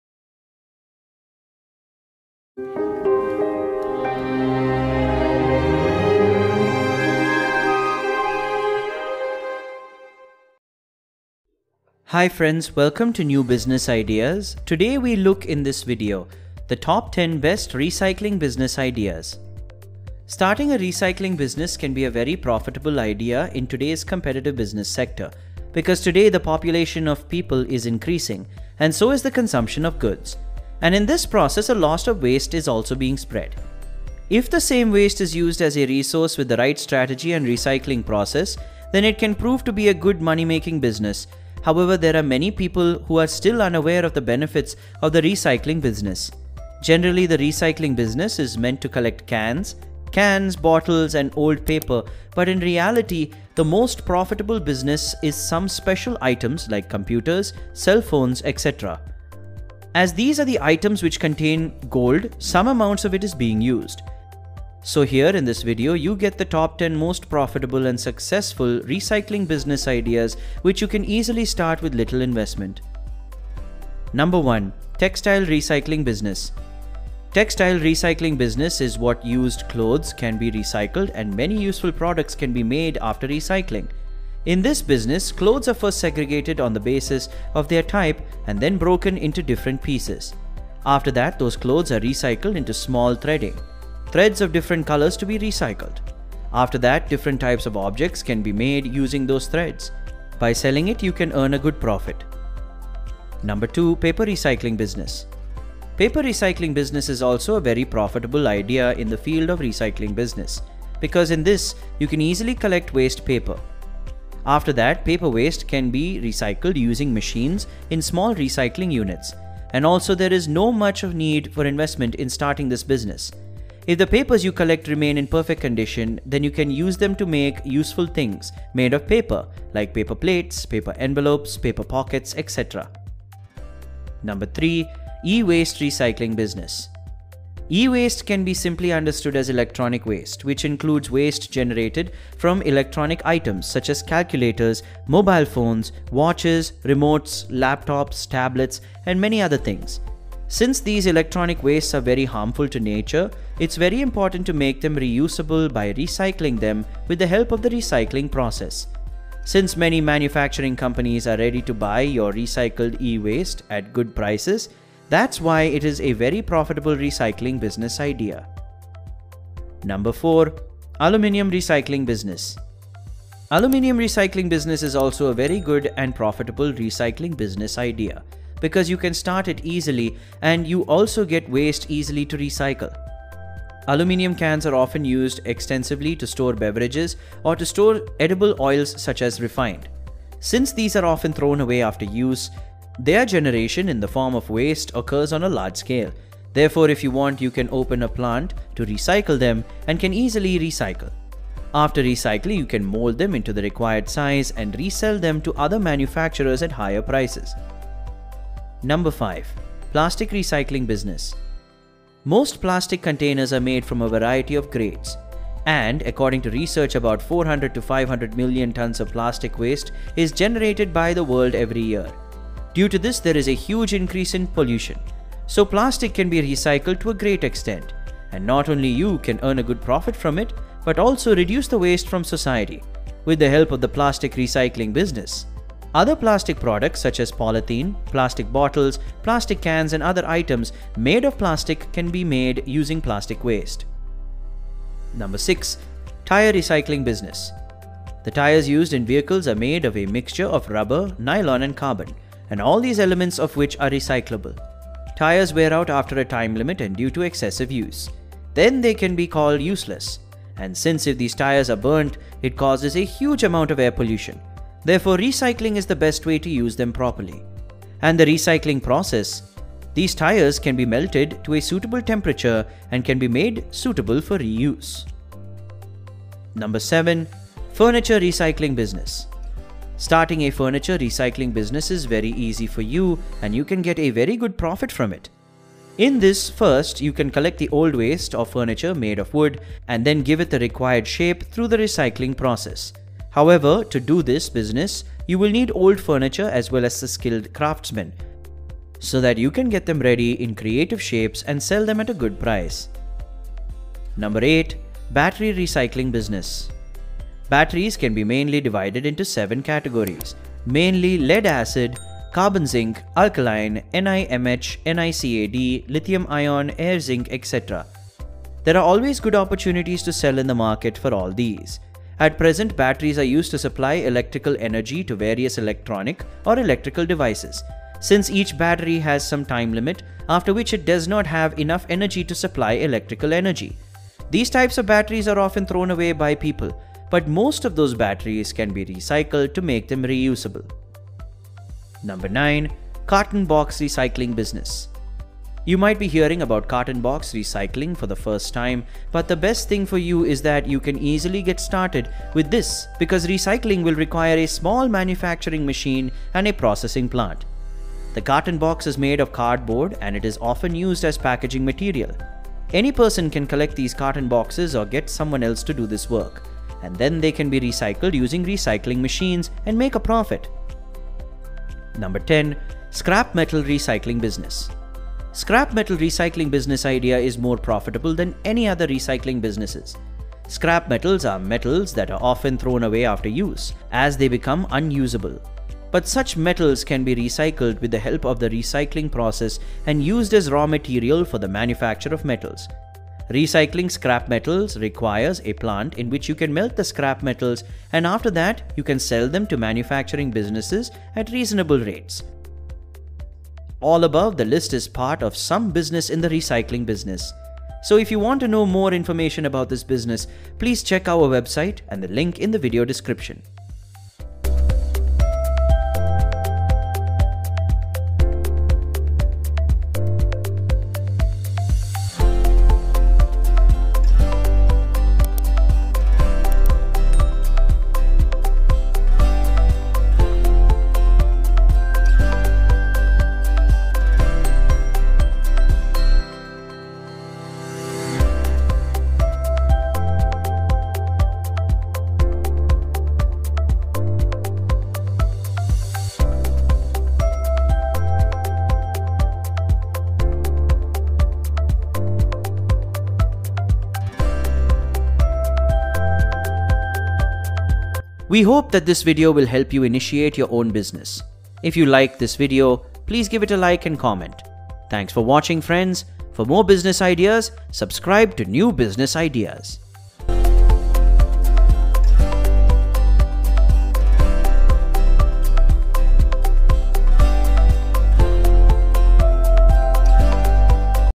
Top 10 Best Recycling Business Ideas - That Are Making a High Profit!
Hai friends, Today we look at this video.

Starting a recycling business can be a very profitable idea in today's competitive business sector. Here in this video, you get the Top 10 most profitable and successful recycling business ideas, which you can easily start with little investment and High Profit.

If you have any questions about these business ideas, please write them in the comments section.